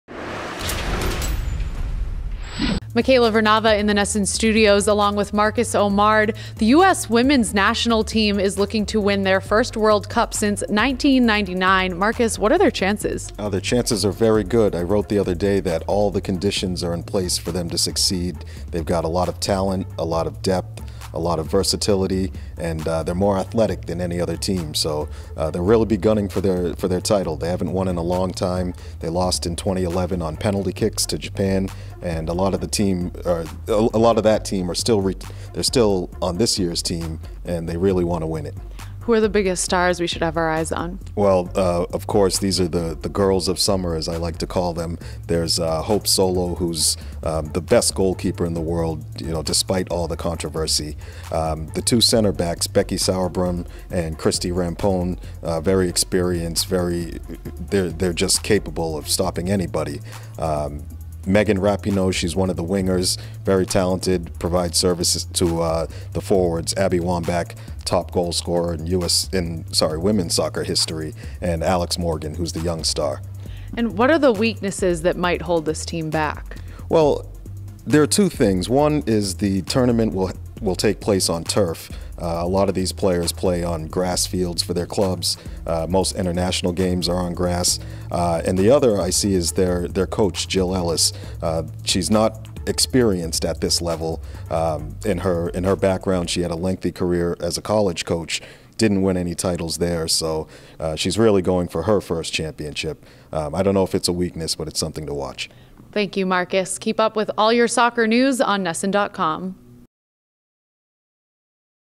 Team USA Poised To Win First FIFA Women's World Cup Since 1999
The Americans haven't won the Women's World Cup since 1999. NESN.com's Marcus O'Mard previews the tournament with Michaela Vernava.

Produced by Andre Khatchaturian